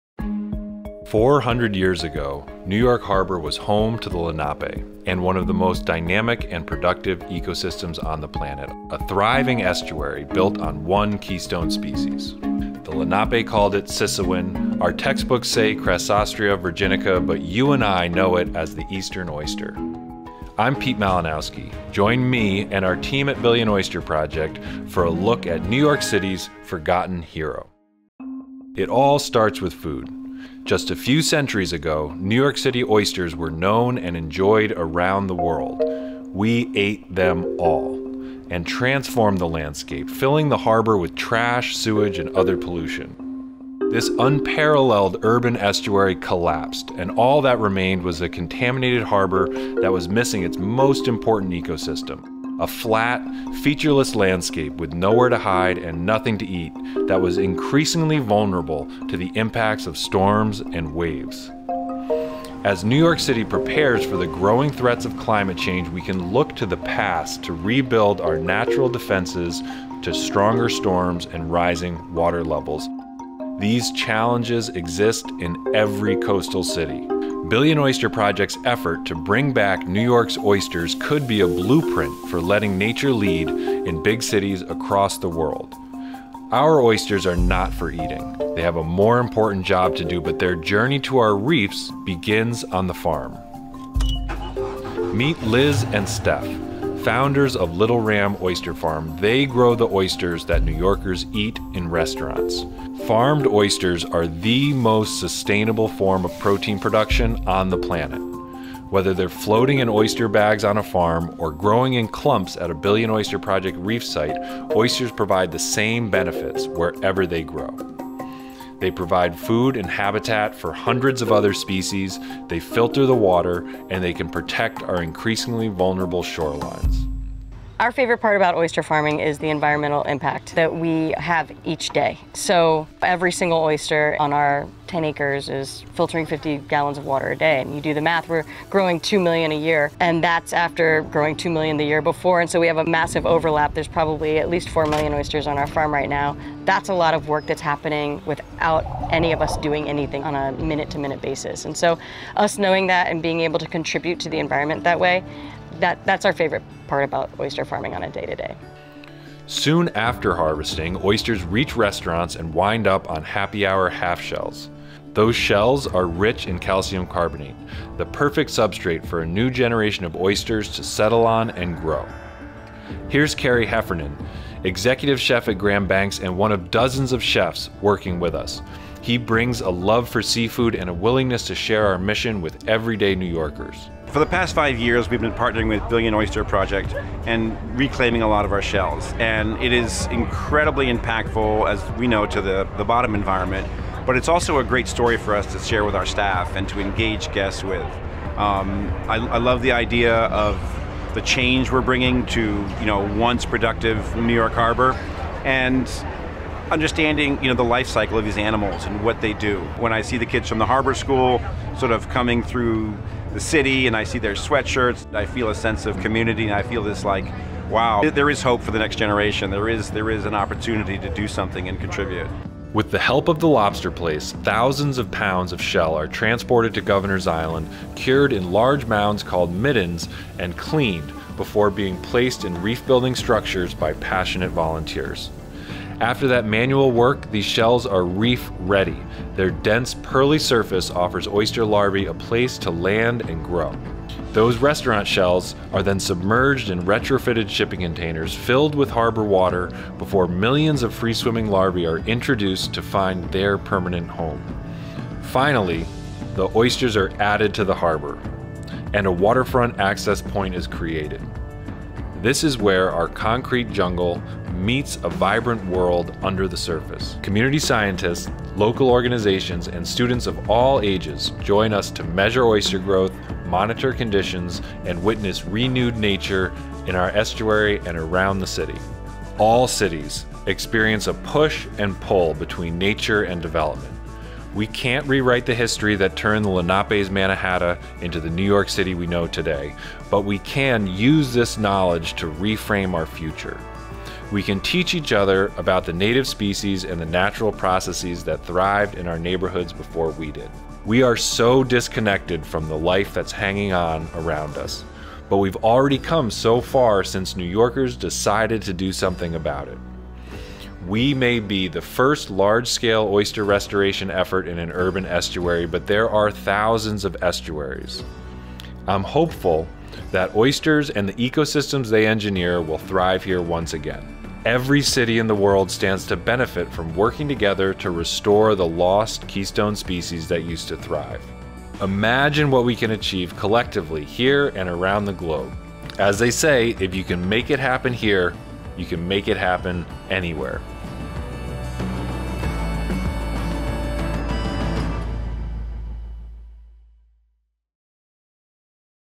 Restaurant to Reef: The Journey to Restore 1 Billion Oysters to New York City
Join us in "Restaurant to Reef", a captivating exploration of Billion Oyster Project, where we trace the extraordinary journey of an oyster from farms, like Little Ram Oyster Co., to your dining table, and eventually to the flourishing oyster reefs of New York Harbor.

On the Farm: Discover how our work begins at conscientious oyster farms, nurturing oysters that not only land on your plates in top New York restaurants but also play a pivotal role in our environmental mission.

On Your Plate: Witness the fascinating cycle as we partner with restaurants to collect oyster shells, transforming them from mere culinary remnants into powerful tools for environmental restoration.

On the Reefs: Immerse yourself in the underwater world of New York Harbor, where these shells form the backbone of new, vibrant oyster reefs. These reefs are not just homes for marine life but also natural barriers protecting our city's coastline.

On Land: Education and Engagement: Learn how the humble oyster becomes a dynamic educational tool, teaching New Yorkers about marine ecology and our interconnectedness with nature, even in an urban landscape.

On The Planet: This journey underscores a vital message: in the face of climate change and environmental degradation, caring for our natural resources is more critical than ever. Our project goes beyond oysters; it's a testament to how each of us can spark a cycle of local stewardship and sustainability.

On Us: You Make the Difference: Remember, an oyster is just an oyster, but this transformative cycle begins with an individual choice – yours and ours. Join the Billion Oyster Project in rekindling our connection with nature, fostering a sustainable environment, and safeguarding New York's iconic waterfront for future generations.

💬 Get Involved: To learn more about the Billion Oyster Project and make an impact in your community, head to billionoysterproject.org.

📣 Share the Journey: Help spread the word about our mission! Big changes can come from the smallest actions.

Thank you to @11thHourRacing @littleramoysterco3566 @NewYorkHarborSchool and so many others!